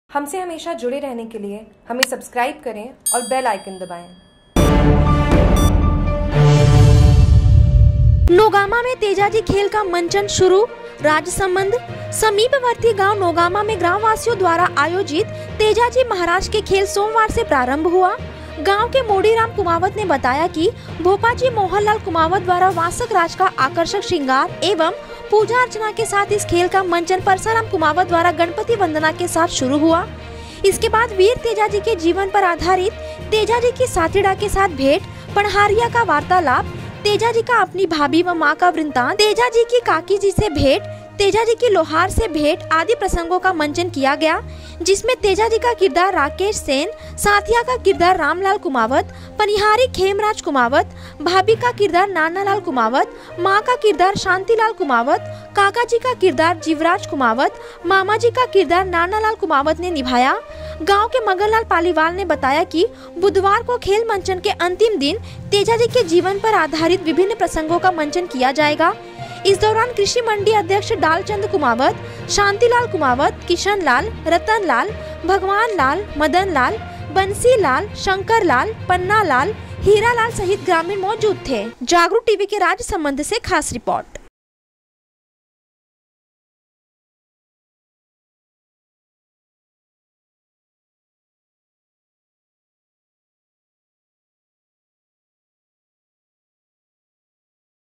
राजसमंद : नोगामा में तेजाजी खेल का मंचन शुरू
समीपवर्ती गांव नोगामा में  ग्राम वासियों द्वारा आयोजित तेजाजी महाराज के खेल सोमवार से प्रारंभ हुआ
गांव के मोडी राम कुमावत  ने बताया की भोपाजी मोहन लाल कुमावत द्वारा वासक राज का आकर्षक श्रंगार एवं पूजा अर्चना के साथ इस खेल का मंचन परसराम कुमावत द्वारा गणपति वंदना के साथ शुरू हुआइसके बाद वीर तेजाजी के जीवन पर आधारित तेजाजी की साथीडा के साथ भेट, पण हारिया का वार्तालाप,तेजाजी का अपनी भाभी व मां का वृतांत,तेजा जी की काका जी से भेंट,तेजाजी की लोहार से भेंट आदि प्रसंगों का मंचन किया गया जिसमें तेजाजी का किरदार राकेश सेन,साथिया का किरदार रामलाल कुमावत,पनिहारी खेमराज कुमावत,भाभी का किरदार नाना लाल कुमावत,मां का किरदार शांति लाल कुमावत,काका जी का किरदार जीवराज कुमावत,मामा जी का किरदार नाना लाल कुमावत ने निभाया
गांव के मगन लाल पालीवाल ने बताया कि बुधवार को खेल मंचन के अंतिम दिन तेजाजी के जीवन पर आधारित विभिन्न प्रसंगों का मंचन किया जाएगा